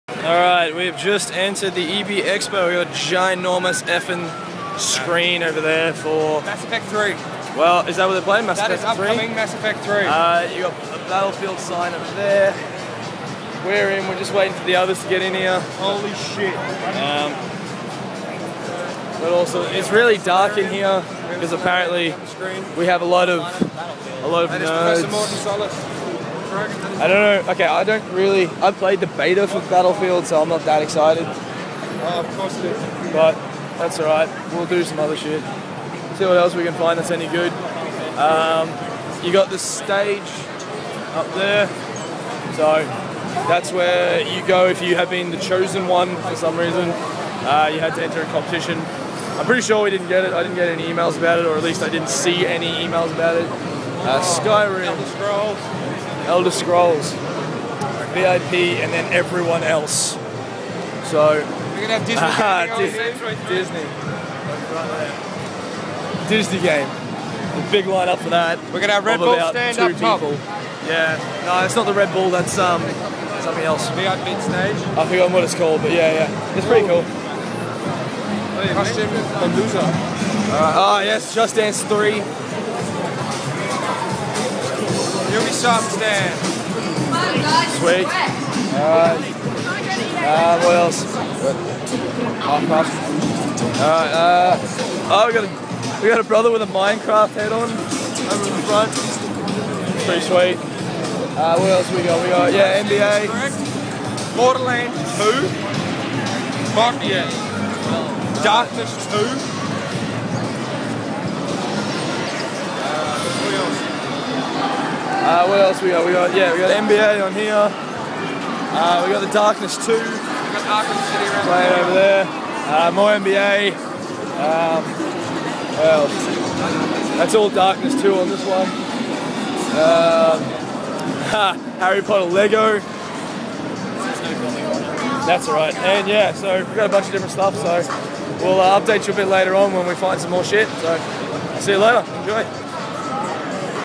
EB EXPO WEEKEND: Entering the expo
TJM1991, DAS1991, MAG1991 and the others go to eb expo and this is us entering the expo :)